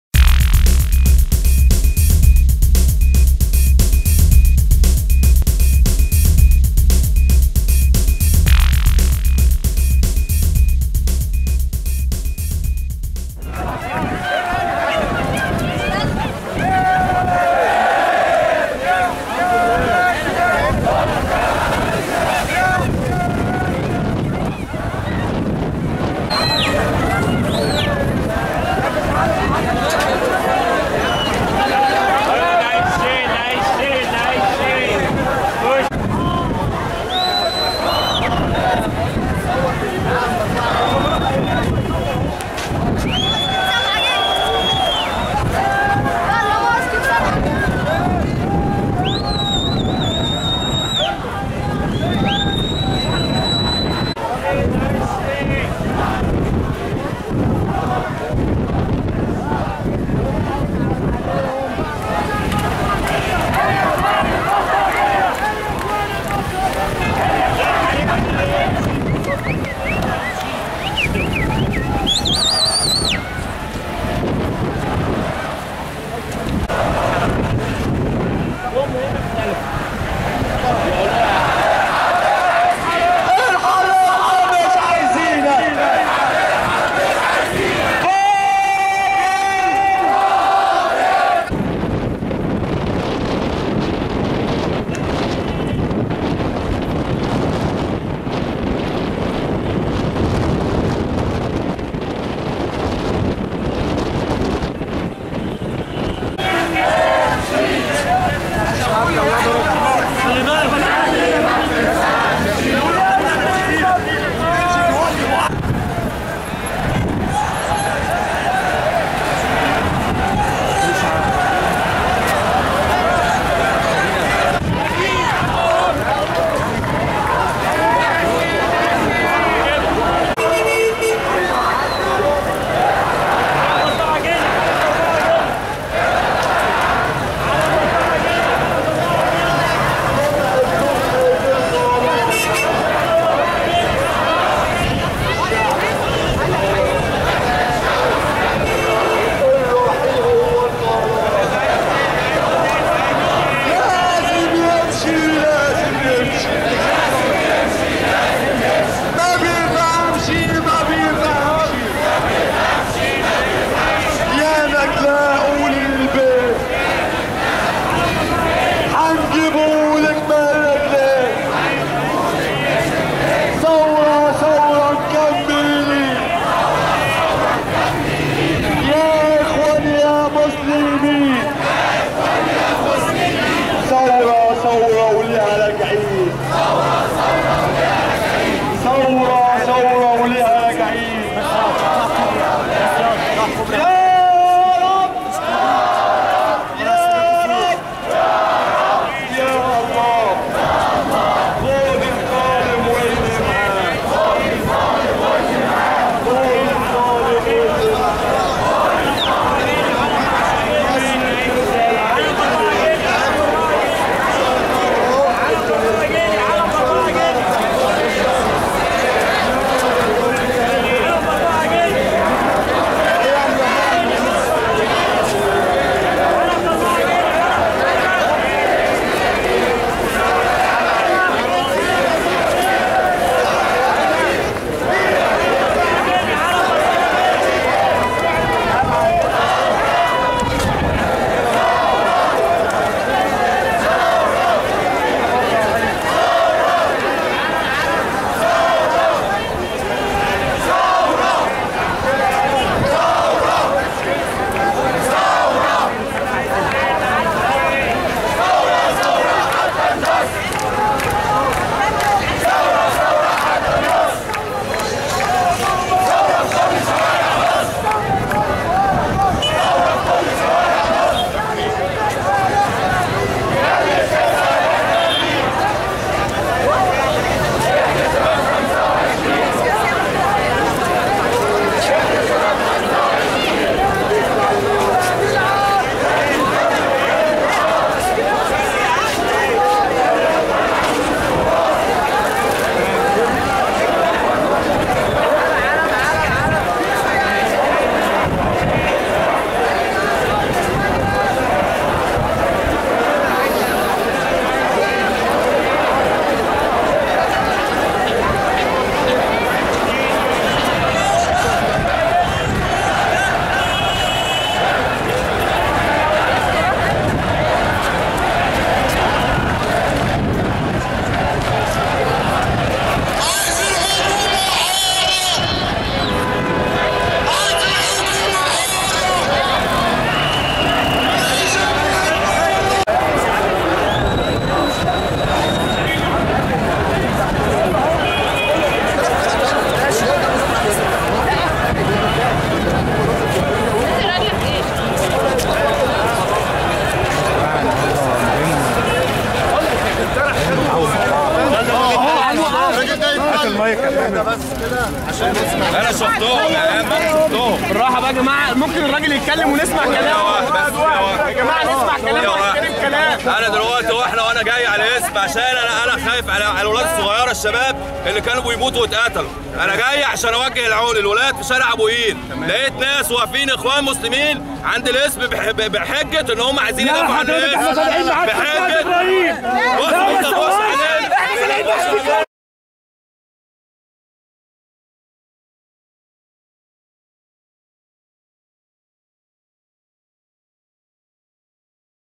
مسيرات الإسكندرية 1/2/2013
نعتذر عن جودة الصوت بسبب شدة الرياح في الكثير من المناطق التى مرت منها المسيره